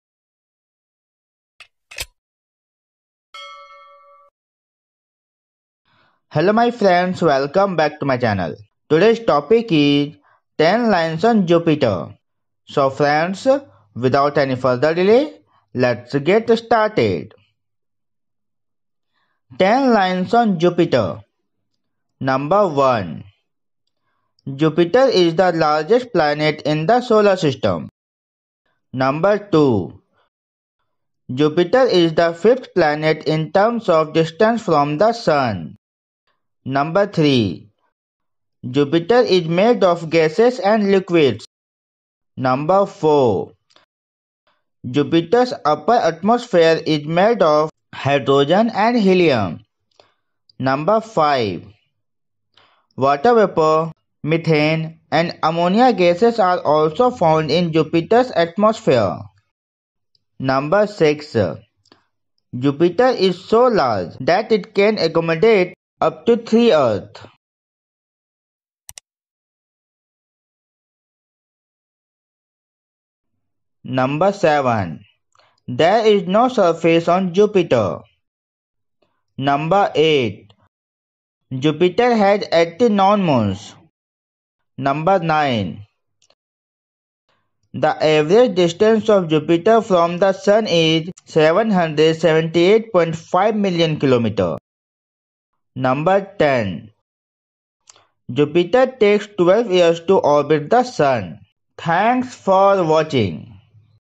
10 Lines on Jupiter In English | Few Lines on Jupiter Planet | Few Sentences About Jupiter Planet
👉 About Video : - Hello my friends today we will learn that how to write 10 Lines on Jupiter In English | Few Lines on Jupiter Planet | Few Sentences About Jupiter Planet